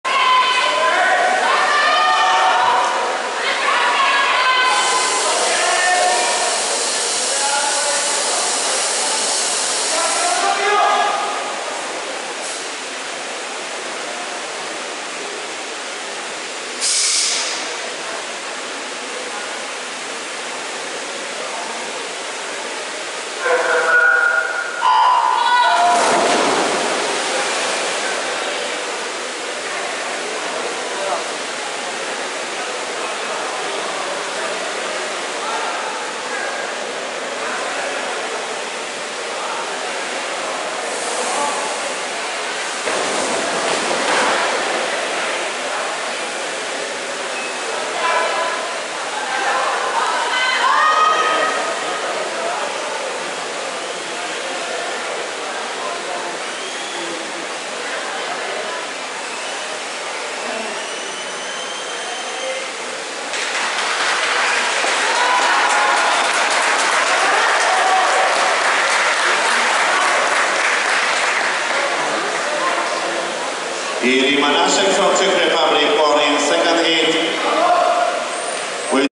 2013 Szczecin 24 AHMET SAYDAM 100 IM
CMAS 2013 Szczecin Finswimming European Junior Championship / CMAS 2013 Szczecin Paletliyüzme Avrupa Gençler Şampiyonası